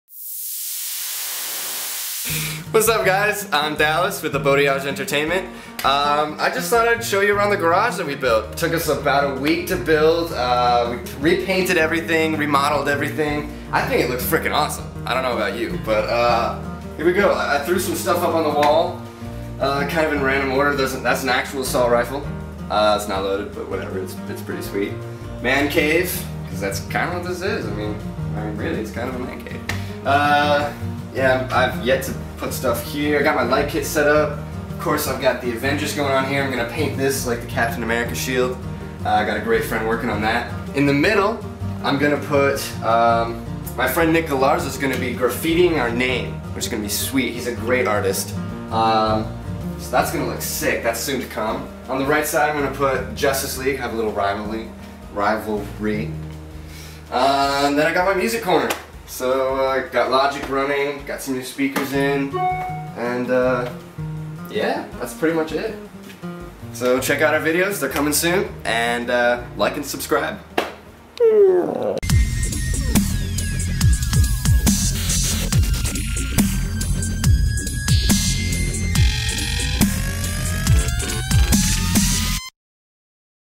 Garage Tour
Check out the awesome garage!!

We took a lot of time and effort to turn this garage into a sweet little film and hang out room!